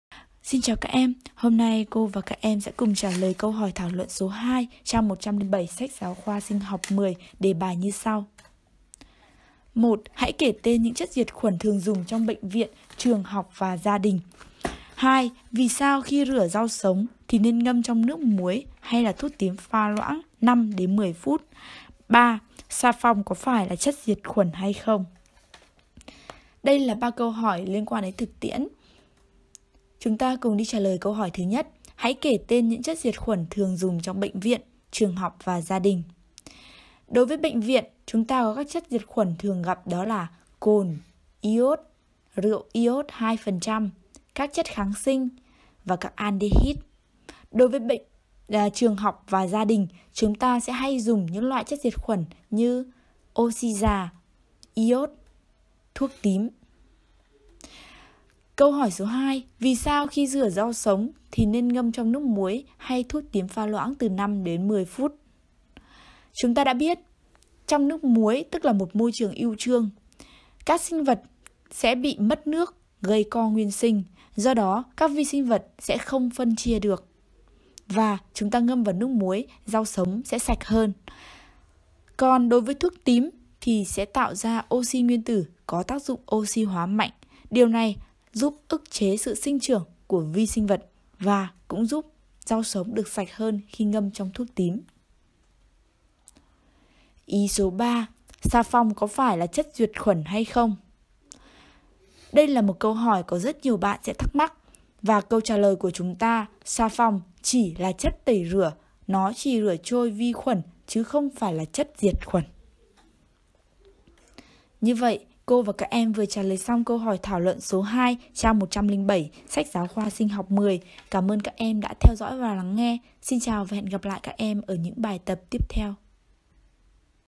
Trả lời câu hỏi thảo luận số 2 Trang 107 SGK Sinh học 10
Trả lời câu hỏi thảo luận số 2 trang 107 SGK Sinh học 10 với lời giải chi tiết, ngắn gọn nhất sẽ giúp các em nắm bắt các kiến thức cơ bản và nâng cao một cách nhanh nhất
Hãy kể những chất diệt khuẩn thường dùng trong bệnh viện, trường học và gia đình.
Vì sao sau khi rửa rau sống nên ngâm trong nước muối hoặc thuốc tím pha loãng 5 – 10 phút?
Xà phòng có phải chất diệt khuẩn hay không? .